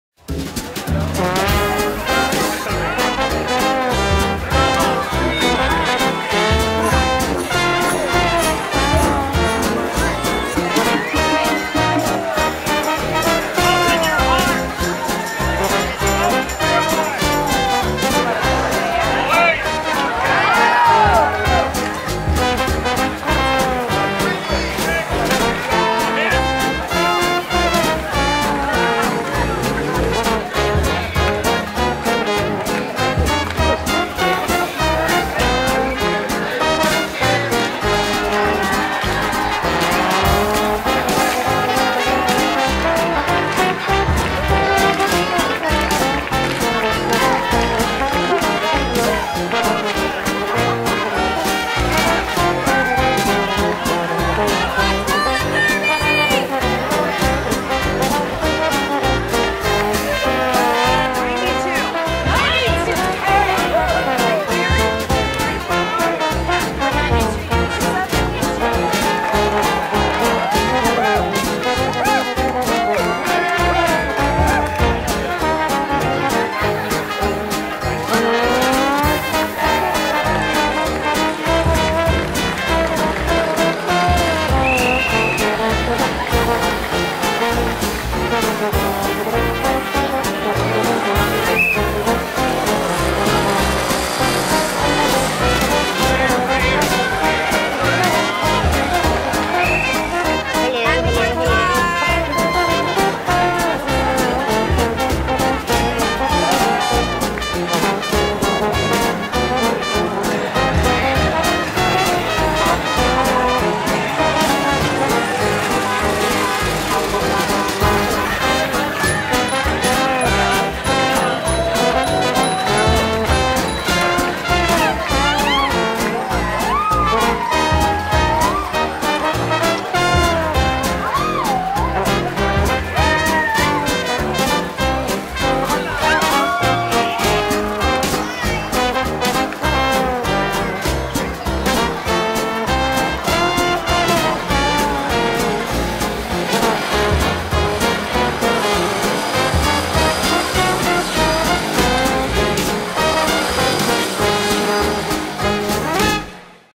Doo Dah Parade - Columbus, OH (2015)
The Doo Dah Parade has been an annual tradition in Columbus, Ohio since 1983.

[ Background music used with permission - licensed from publisher. ]